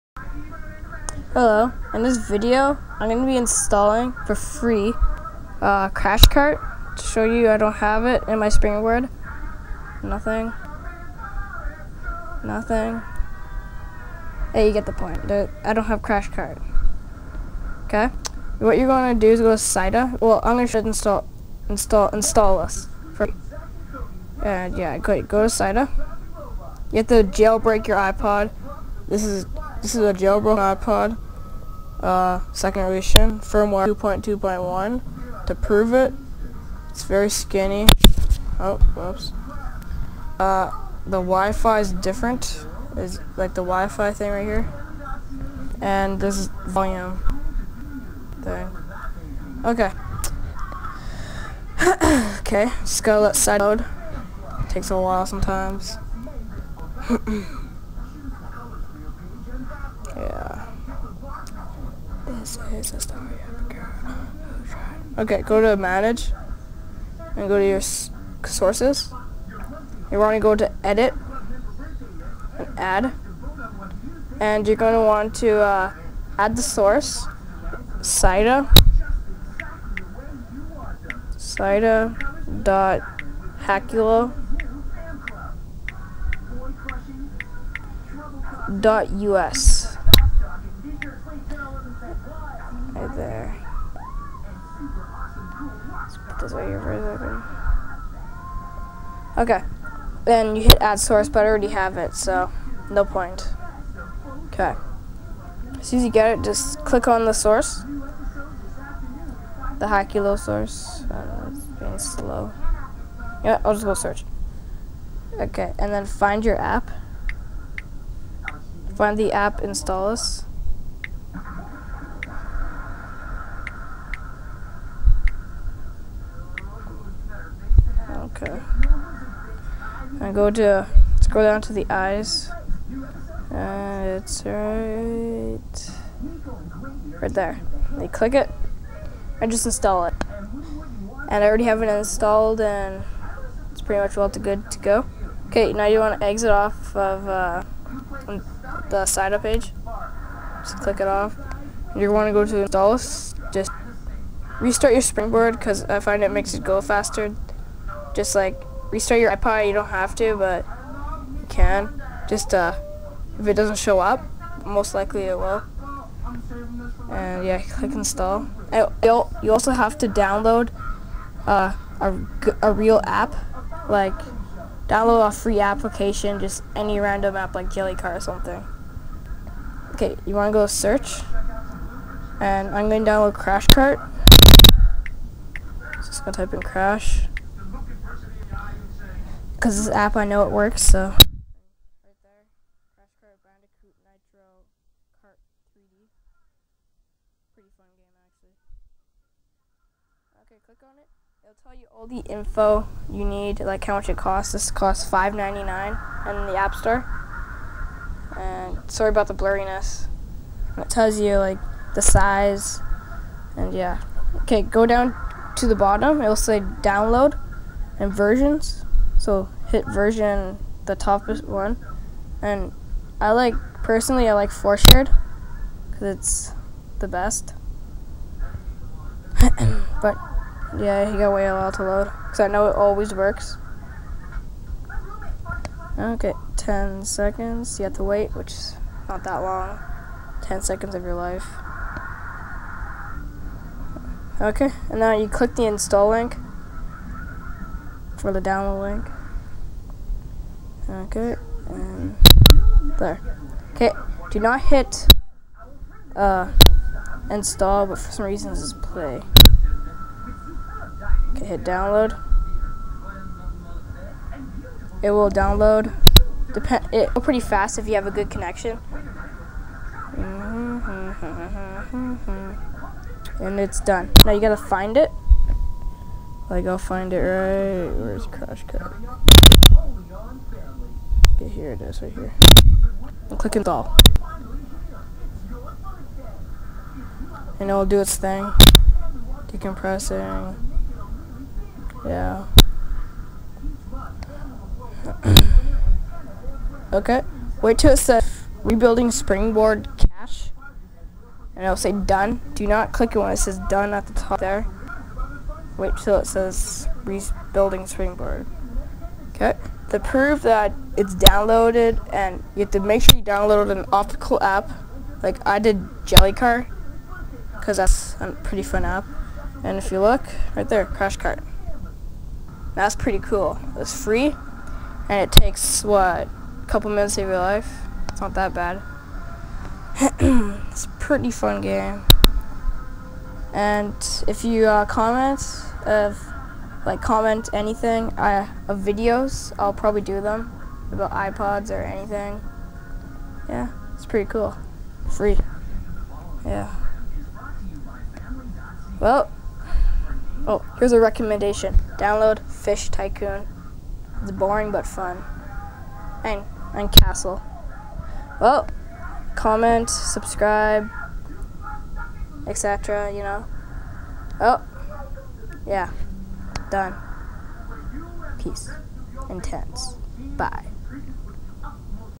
how to get free paid apps on ipod touch 1g,2g,3g (no computer)
your ipod has to be jailbroken  cydia.hackulo.us